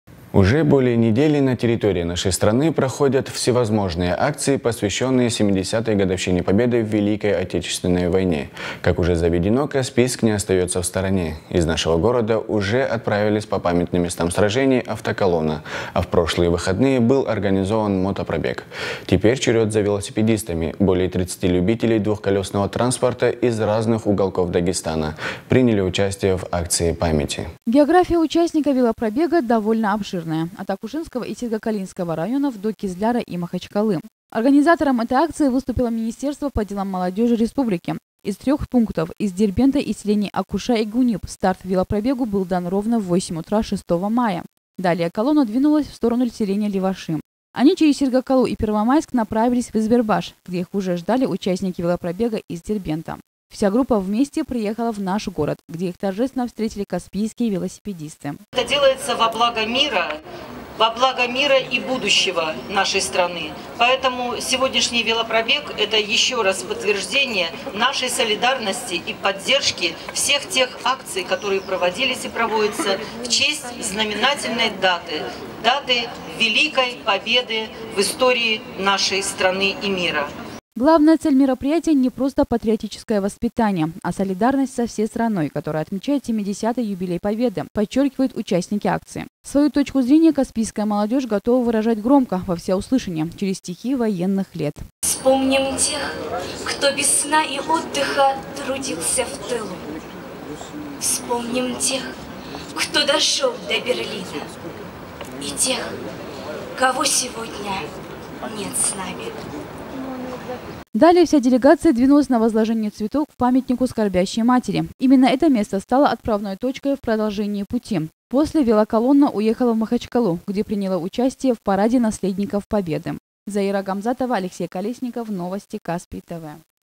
Велопробег, приуроченный 70-ой годовщине победы
Велопробег, приуроченный 70-ой годовщине победы, собрал в Каспийске представителей многих городов и районов республики.

Уже более недели на территории нашей страны проходят всевозможные акции, посвященные 70-ой годовщине победы в великой отечественной войне. Как уже заведено, Каспийск не остается в стороне. Из нашего города уже отправились по памятным местам сражений автоколонна, а в прошлые выходные был организован мотопробег. Теперь черед за велосипедистами. Более 30 любителей двухколесного транспорта из разных уголков Дагестана приняли участие в акции памяти.

География участников велопробега довольно обширная от Акушинского и Сергокалинского районов до Кизляра и Махачкалы. Организатором этой акции выступило министерство по делам молодежи республики. Из трех пунктов: из Дербента и селений Акуша и Гуниб старт велопробегу был дан ровно 08:00 6 мая, далее колонна двинулась в сторону селения Леваши. Они через Сергокалу и Первомайск направились в Избербаш, где их уже ждали участники велопробега из Дербента. Вся группа вместе приехали в наш город. Где их торжественно встретили наши велосипедисты.

Главная цель мероприятия не просто патриотическое воспитание, а солидарность со всей страной, которая отмечает 70-летний юбилей победы, подчеркивают участники акции. Свою точку зрения каспийская молодежь готова выражать громко, во всеуслышание через стихи военных лет

Далее вся делегация двинулась на возложение цветов к памятнику скорбящей матери. Именно это место стало отправной точкой в продолжение пути. После велоколонна уехала в Махачкалу, где примет участие в параде наследников Победы.

Заира Гамзатова Алексей Колесников новости Каспий-тв